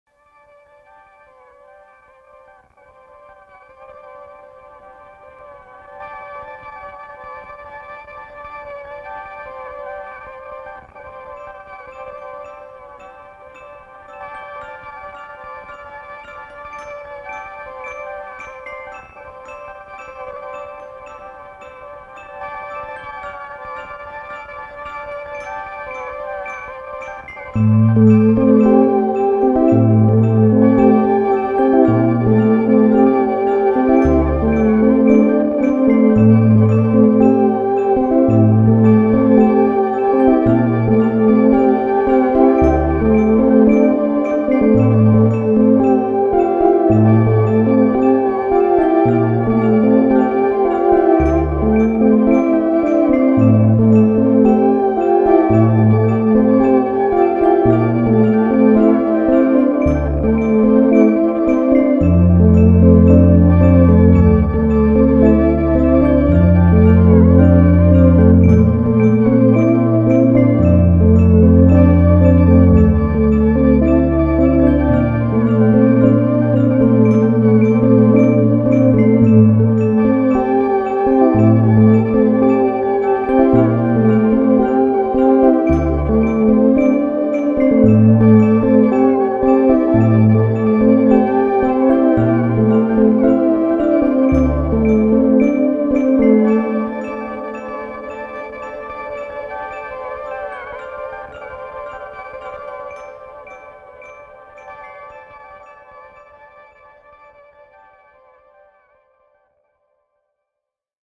070121Jamuary
I'm trying to participate in 'Jamuary' this year by creating a little something each day. I'll be uploading them as i go.